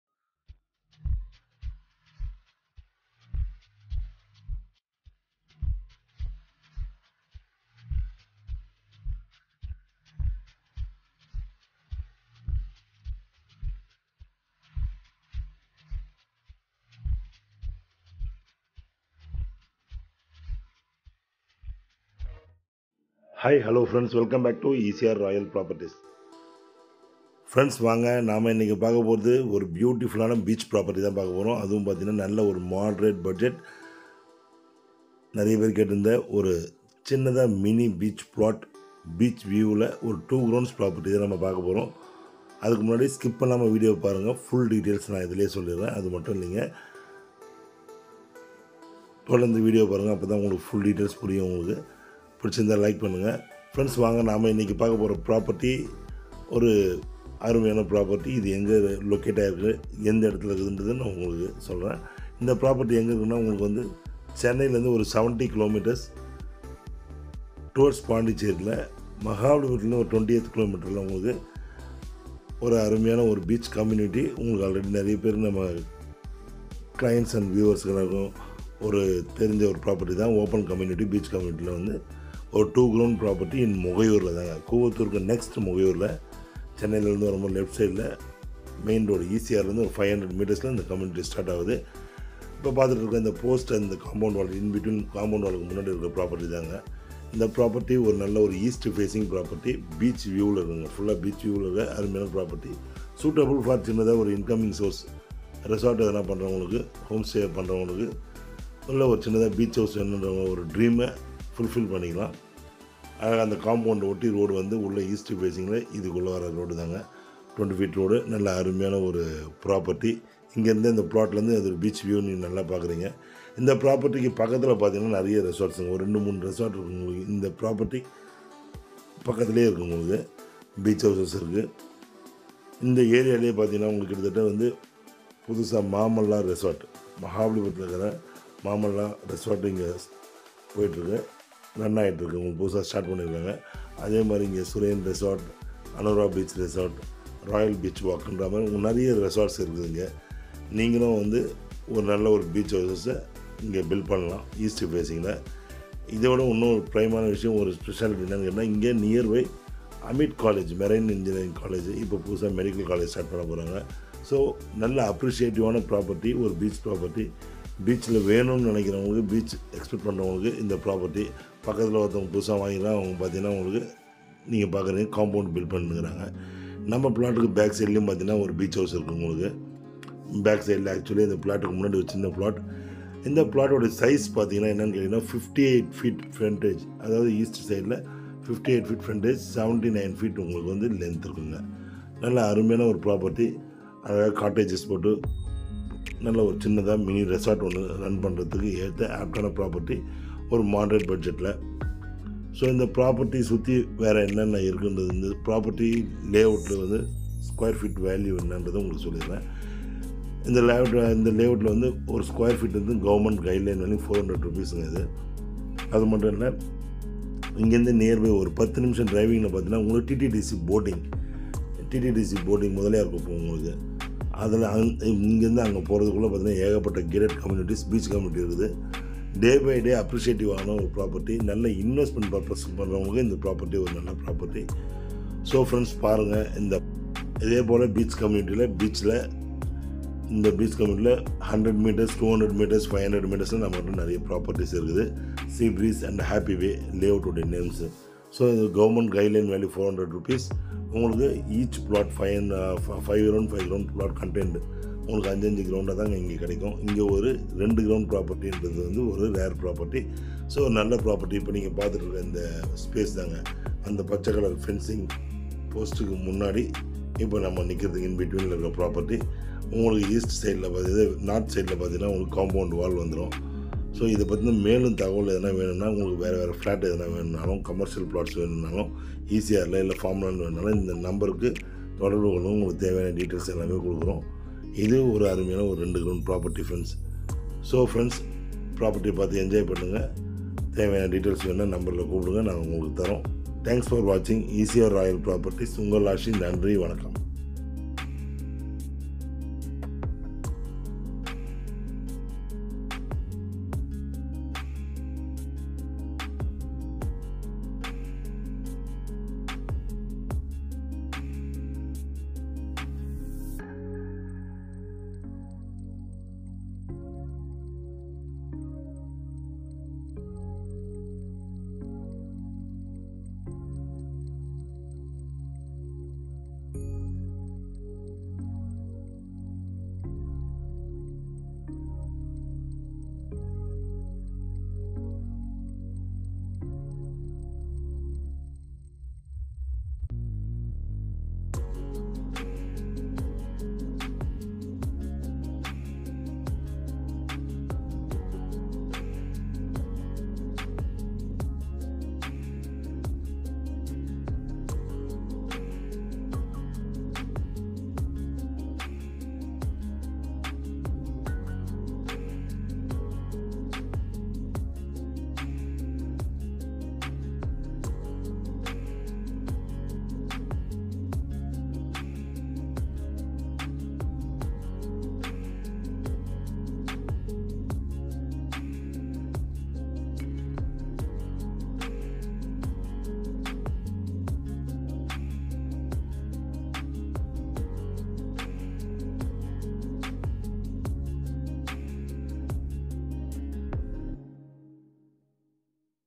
ID 1118: SOLD OUT 12 Lakh ECR BEACH FARMLAND FOR SALE MUGAIYUR 🌴2 GRND EAST FACE🌴🌴RESORT🌴MR.ASHI
A WONDERFUL BEACH PROPERTY ECR
NEAR MUGAIYUR, KOOVATHUR
NEAR AMET MARINE COLLEGE
NEAR MAMALLA RESORT
500 MTRS FROM 8 WAY ECR
NEAR RESIDENTIAL AREA
NEAR VILLAGE RESORT & RESTAURANT
TTDC BOATING RAINDROP
NATURED AMBIENCE ATMOSPHERE
PURE SANDY SOIL PROPERTY
SWEET DRINKING WATER SOURCE
FOR SUITABLE BEACH RESORT⛵ RESTAURANT
SUITABLE FOR PEACEFUL HOME STAY
EAST FACE
AREA: 2 GRND (R) 9 4400 SQFT
PRICE: RS.25 LAKHS
GUIDELINE VALUE: RS.400/SQFT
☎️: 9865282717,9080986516

Service charges :2%